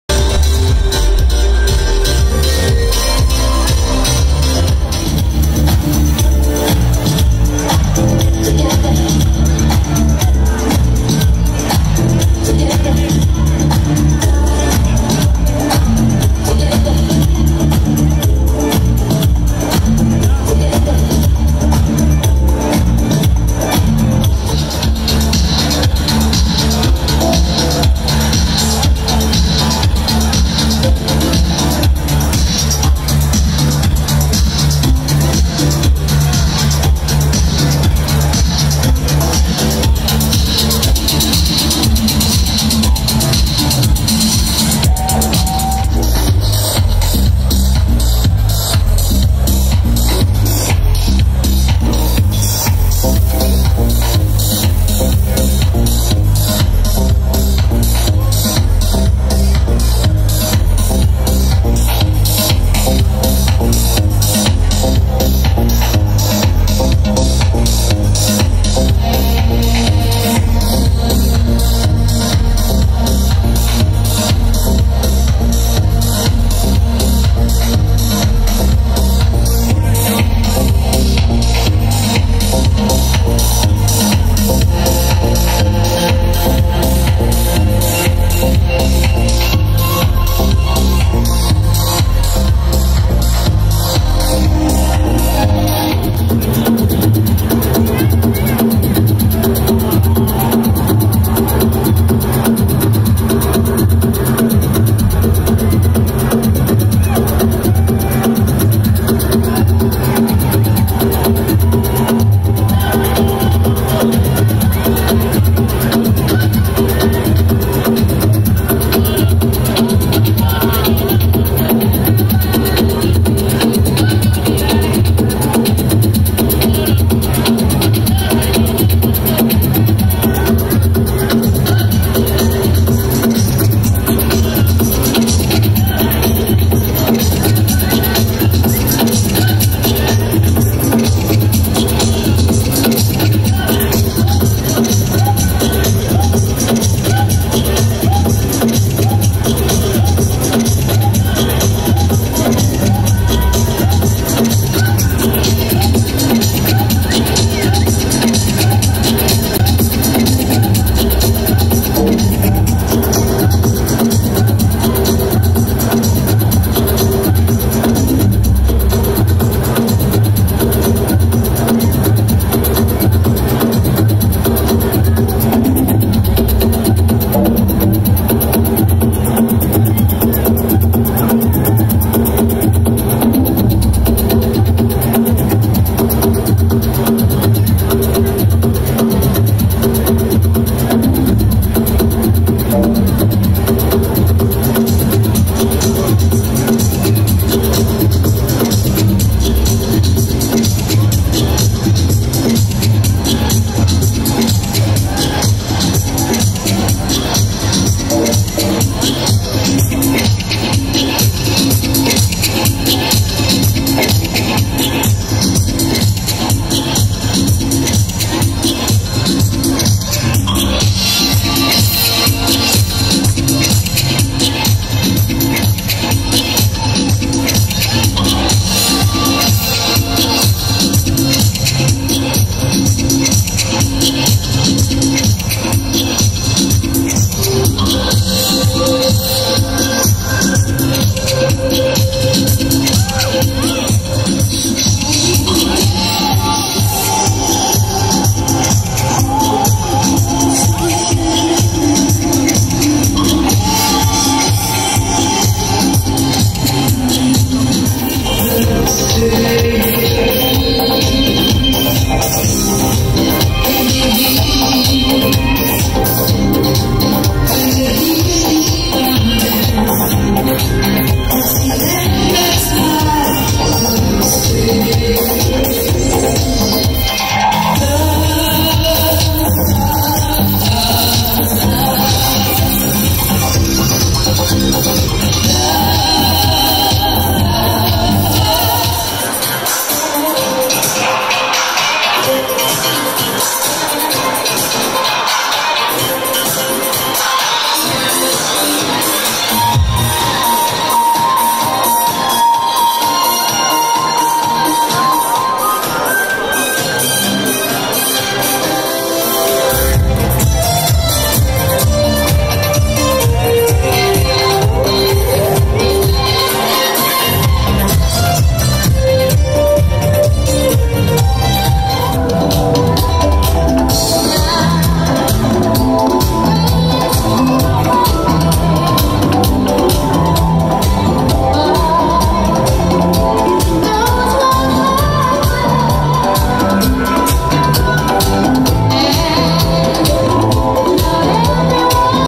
A night with DJ Harvey at Marktkantine April 2019
DJ Harvey back in Amsterdam, unfortunately I didn't bring my Nokia PureView, the sound of the Pixel 3 is not the best but obviously now the image works out better.